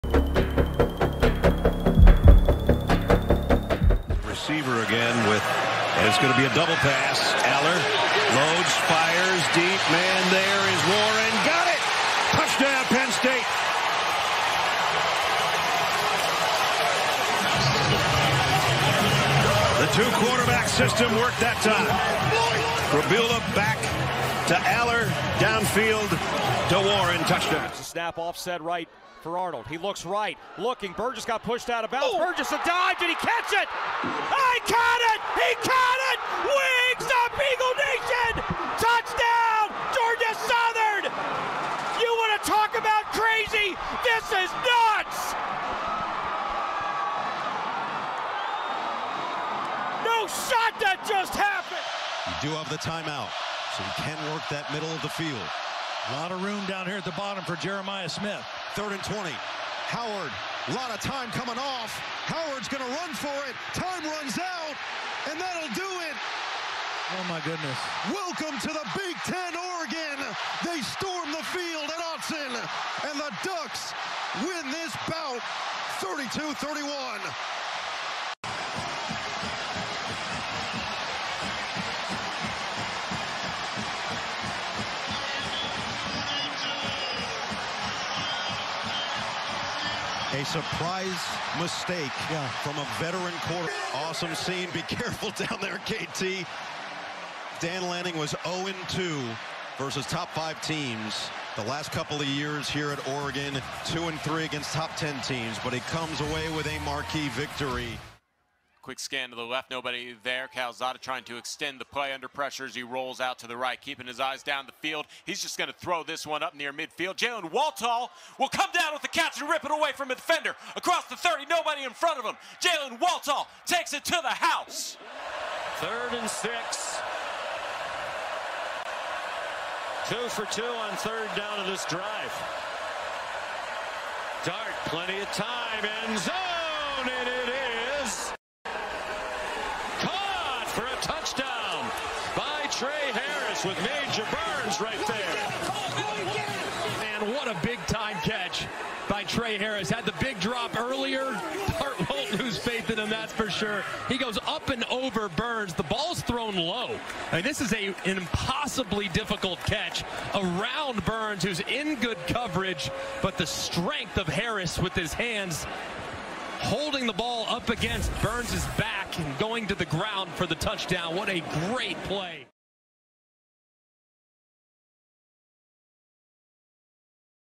Top 5 Plays from Week 7 of College Football 2024
These are the 5 best plays/moments from Week 7 of the college football season!

Inspired by channels like Bordeaux, PerramCrowe, Bengal, and SG1 Sports.